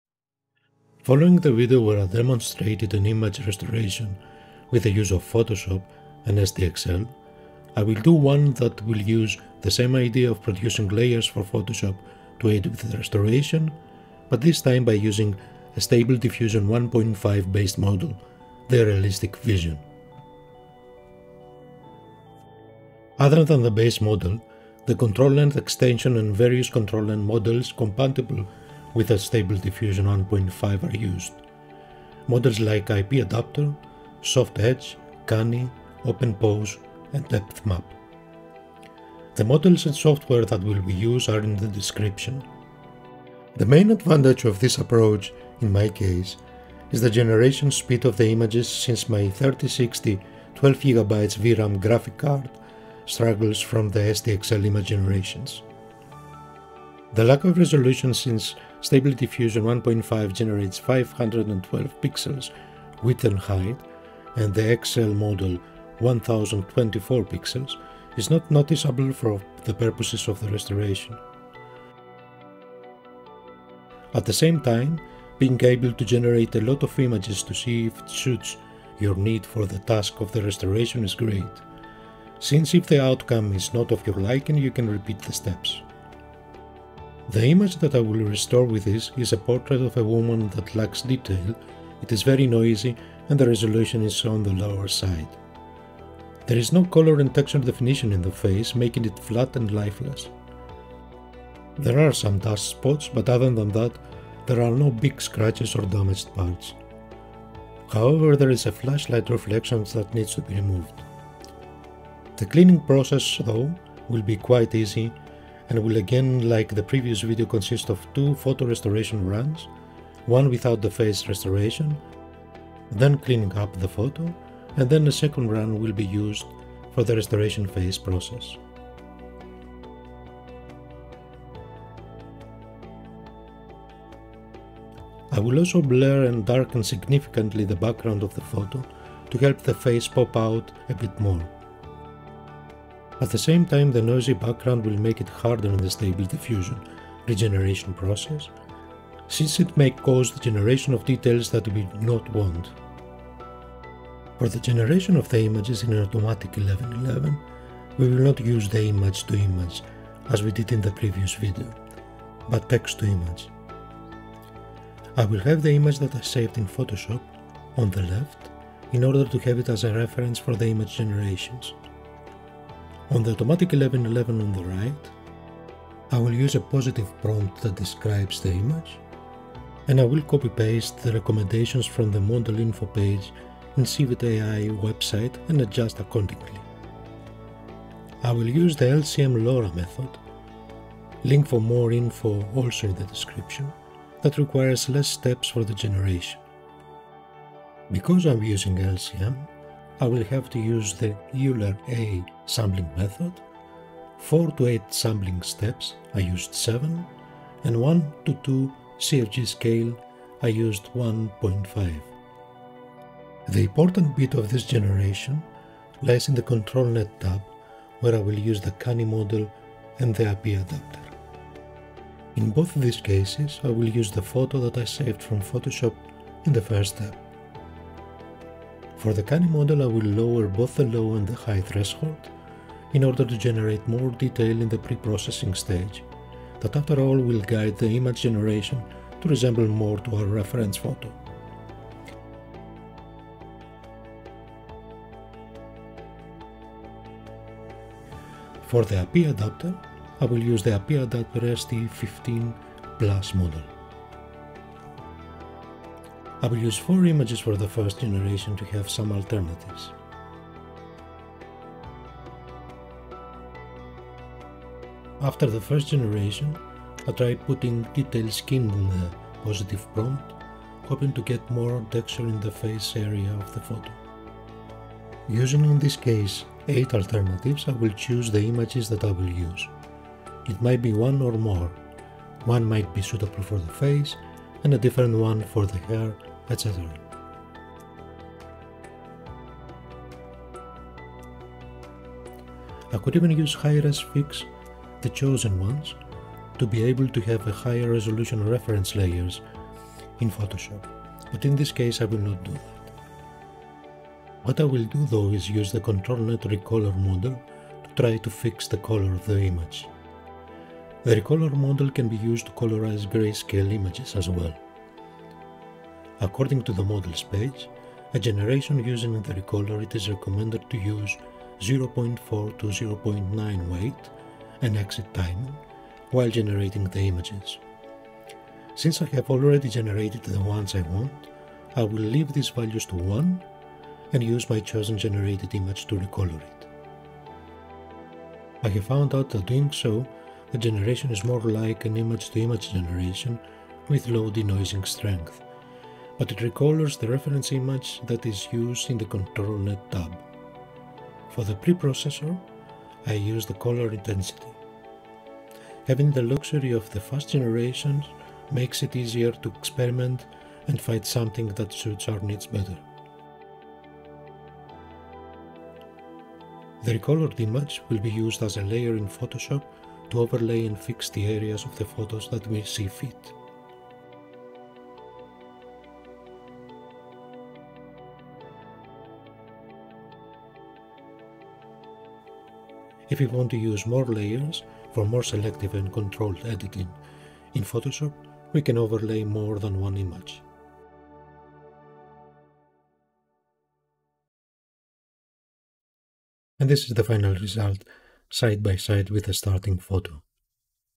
Photo restoration / reimagining / recoloring using Photoshop and RealisticVision SD1.5 model
00:00 - 00:42 Intro
00:42 - 01:26 Why use SD1.5 instead of SDXL
01:27 - 02:27 Editing in Photoshop
02:28 - 03:13 AUTOMATIC 1111 Positive and Negative Prompts
03:14 - 03:32 AUTOMATIC 1111 LCM generation settings
03:33 - 05:03 AUTOMATIC 1111 Controlnet Canny and IP adapter
05:04 - 06:04 AUTOMATIC 1111 Controlnet Recoloring
06:05 - 06:40 Finishing in Photoshop

Links
Automatic 1111

Controlnet Models
Soft Edge, Cunny, Open Pose and Depth Map

Recolor Controlnet Model
Info